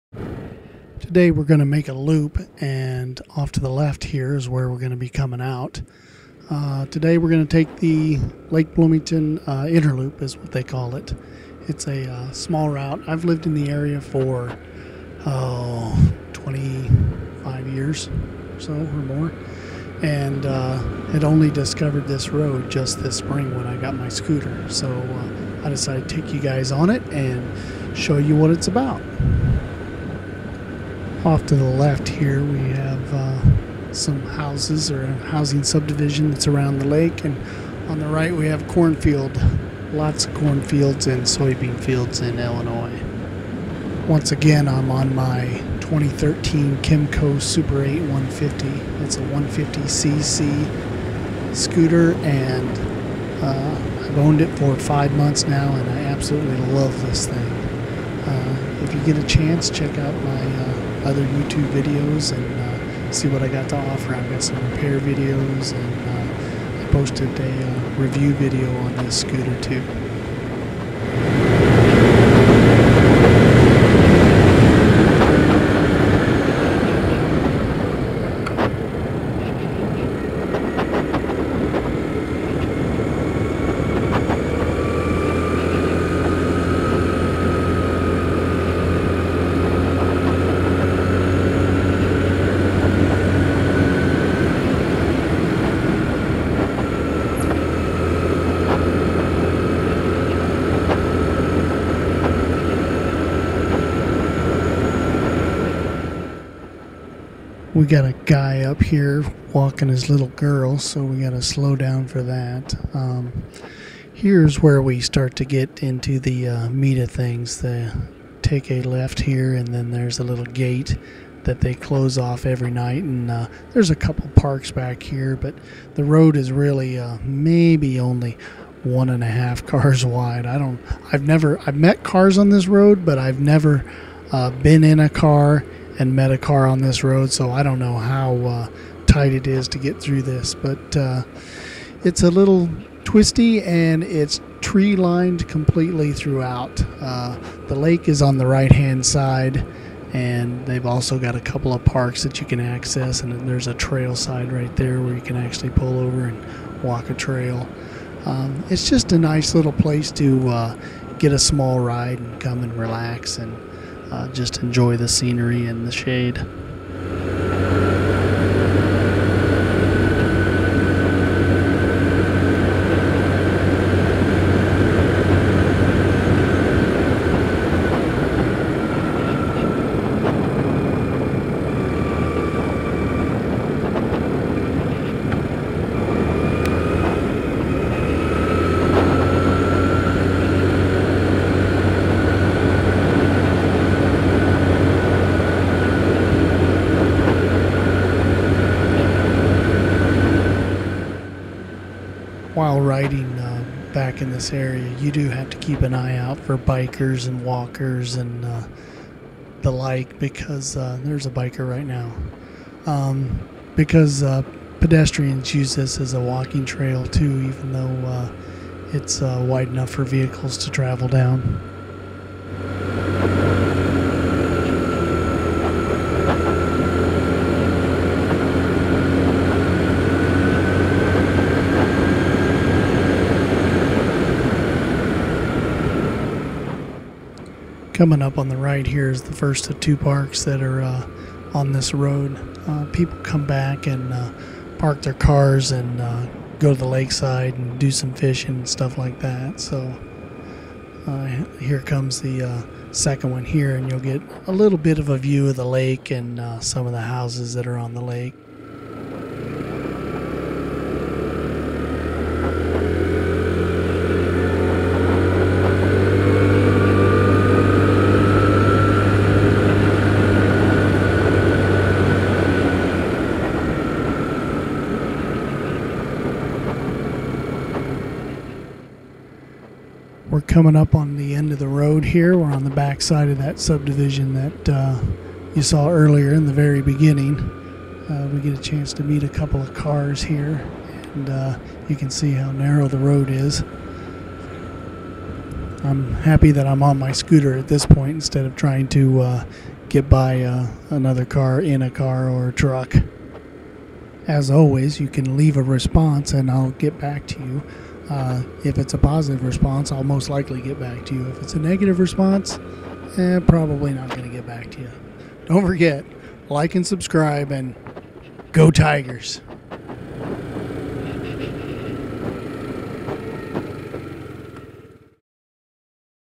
Wayne's vlog #63 - Inner Loop Moto vlog 2
The first of two rides around Lake Bloomington.